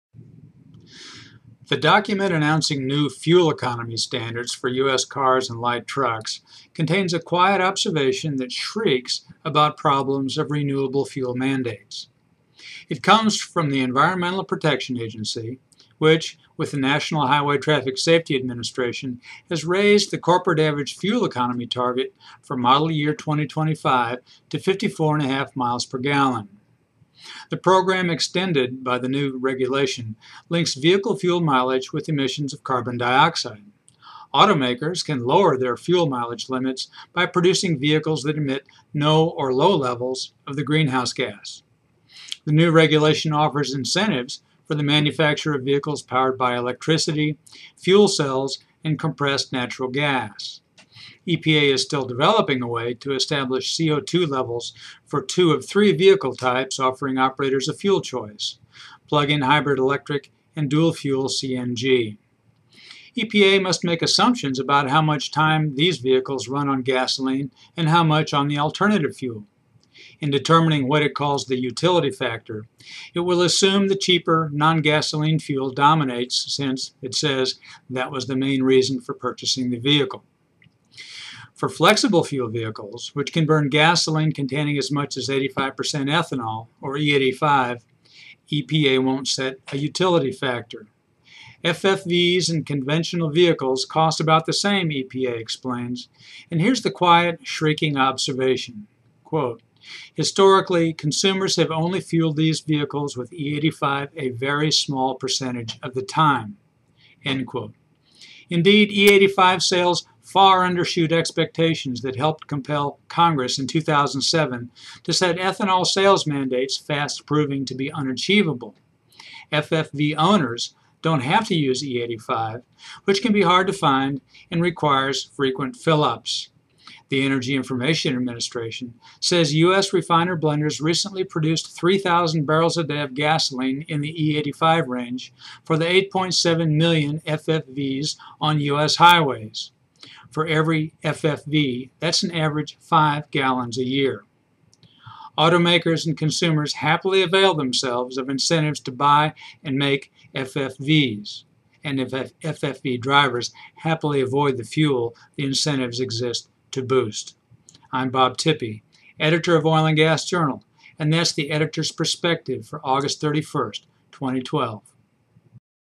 EPA fuel economy announcement notes problems with E85
The document announcing new fuel-economy standards for US cars and light trucks contains a quiet observation that shrieks about problems of renewable-fuel mandates.